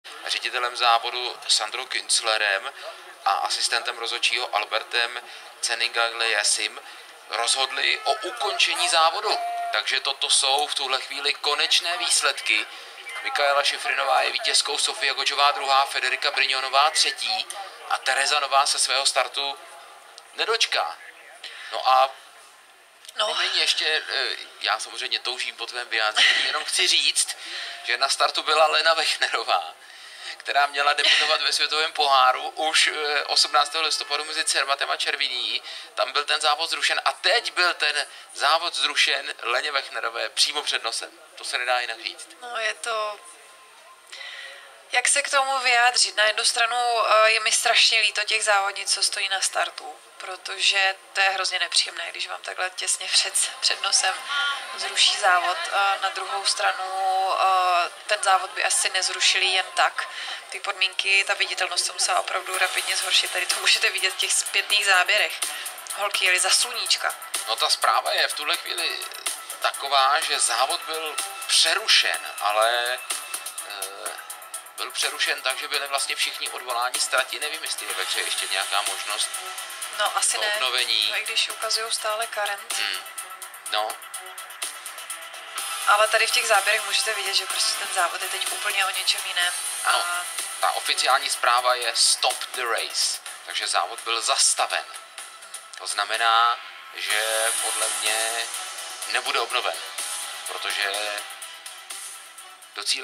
Women's Downhill - Award Ceremony - St. Moritz SUI - 2023
1. Mikaela SHIFFRIN
2. Sofia GOGGIA
3. Federica BRIGNONE
FIS Alpine Ski World Cup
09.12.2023